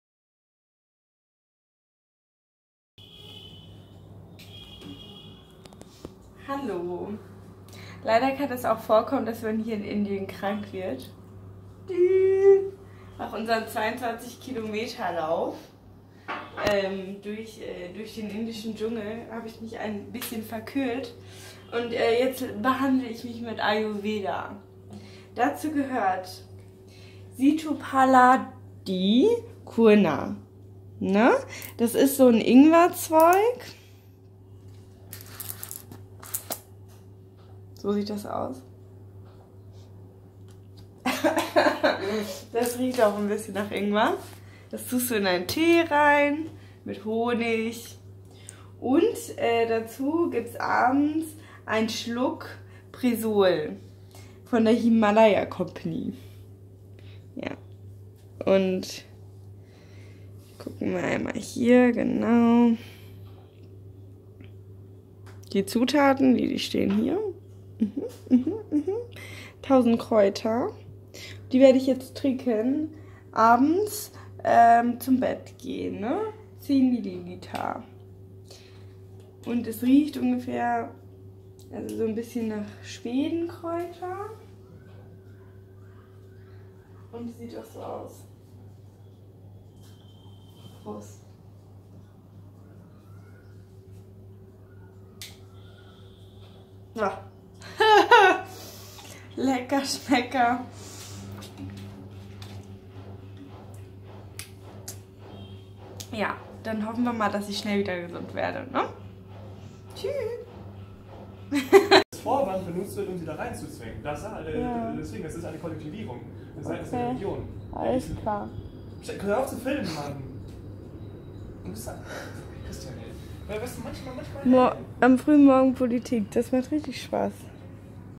Stephie braucht ayurvedische Medizin in Indien
Stephie ist schwer krank und benötigt dringend  ayurvedische Medizin in Indien. Wird die Medizin sie retten bevor es noch schlimmer wird?